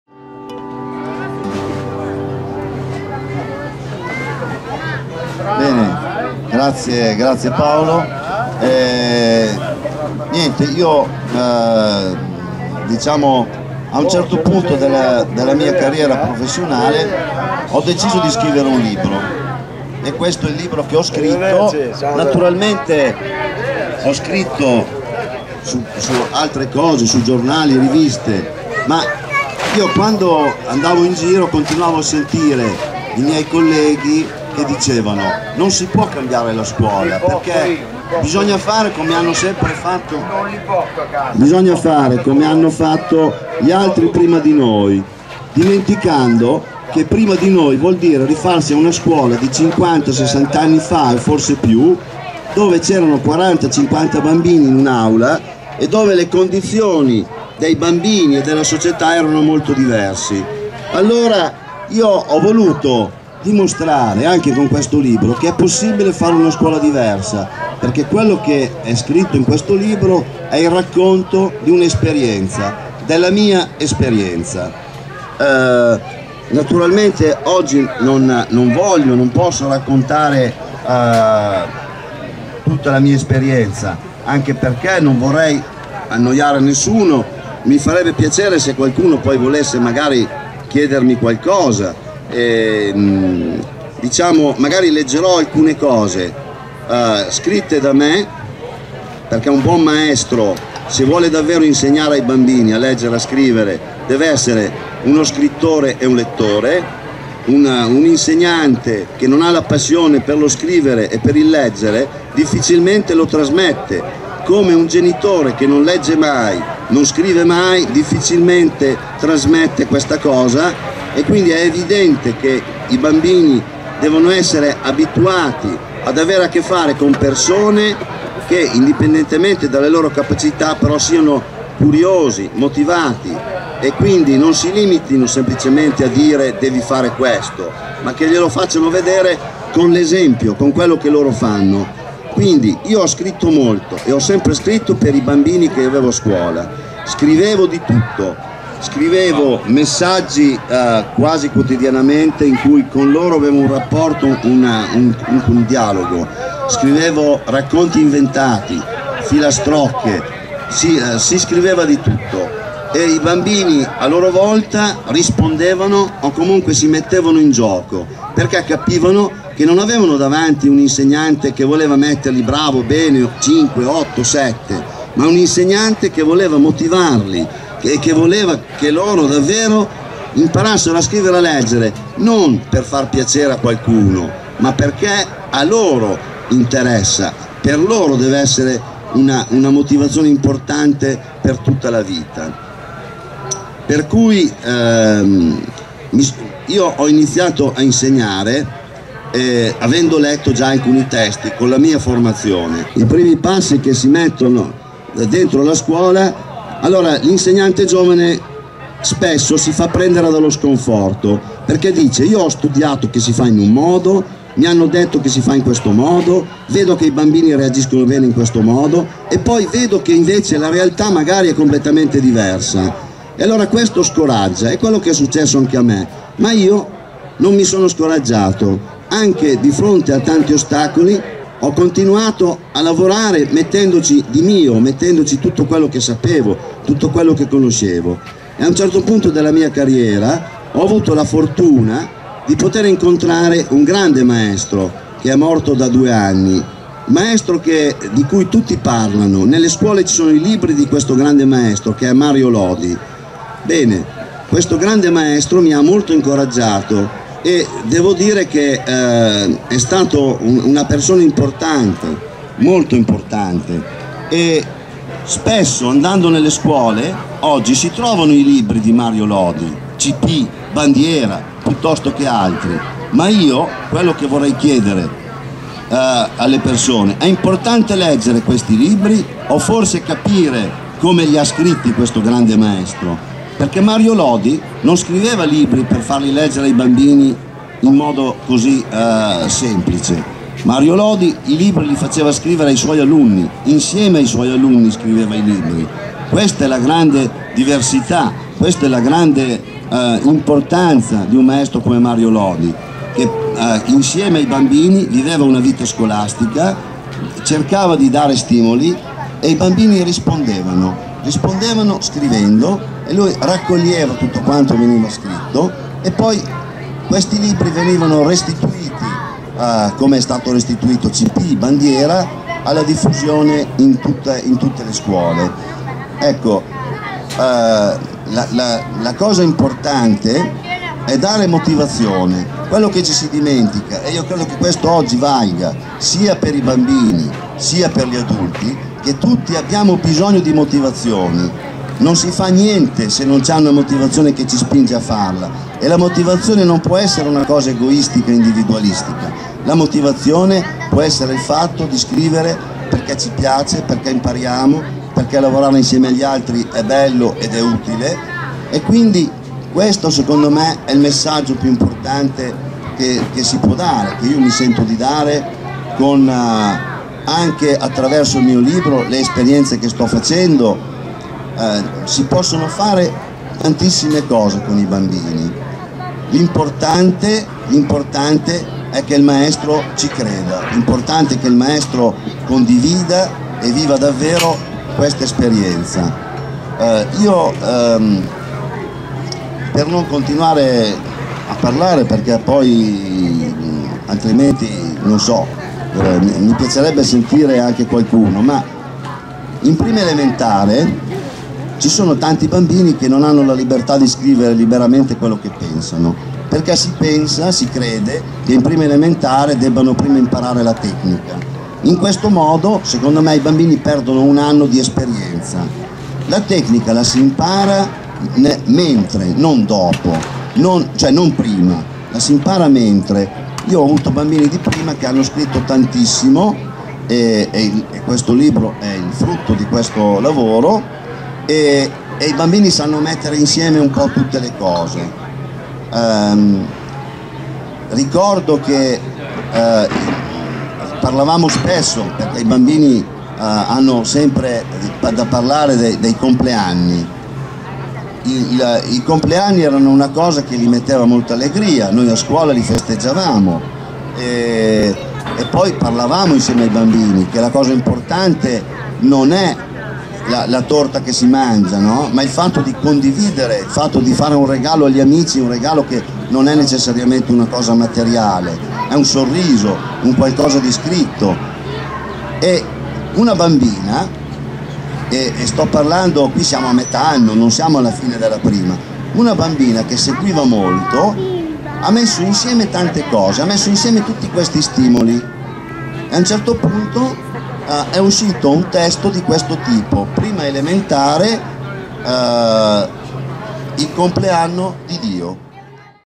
ROBERTO LOVATTINI PRESENTAZIONE LIBRO PARLO 25.09.2016 DI ME VIDEO 2
Presentazione del libro parlo di me di Roberto Lovattini in occasione della sagra dell'anatra di Valconasso  2016